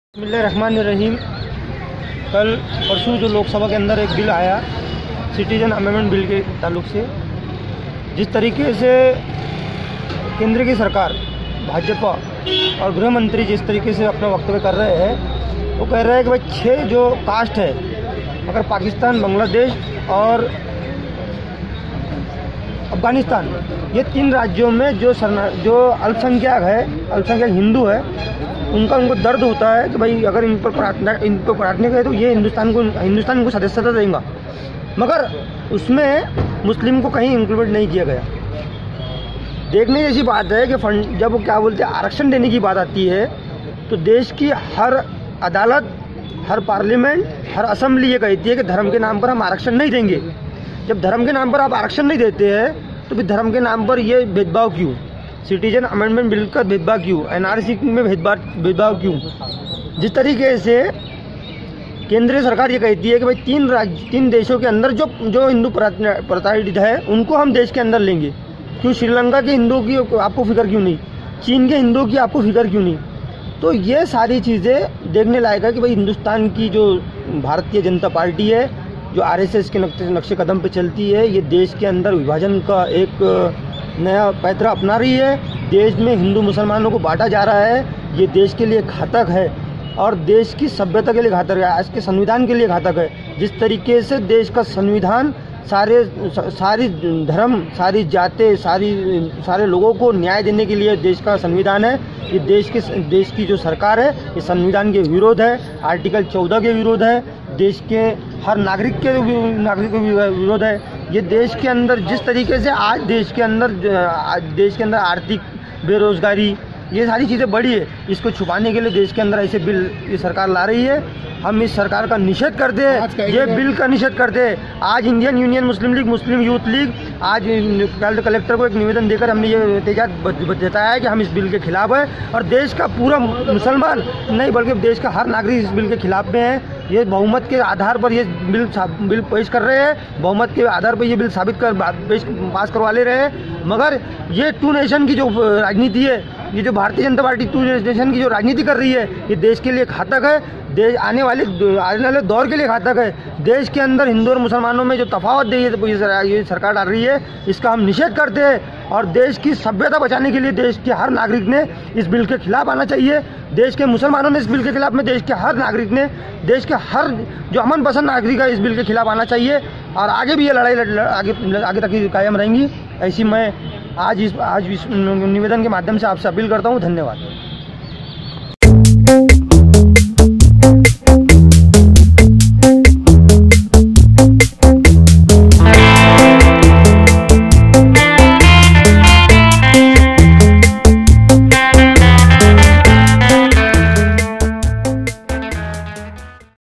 Indian Union Muslim Legue प्रदेशाध्यक्ष Imran Ashrafi CAB/NRC पर प्रोटेसट Maharashtra
Indian Union Muslim Legue युथ प्रदेशाध्यक्ष Imran Ashrafi CAB/NRC पर प्रोटेसट Maharasht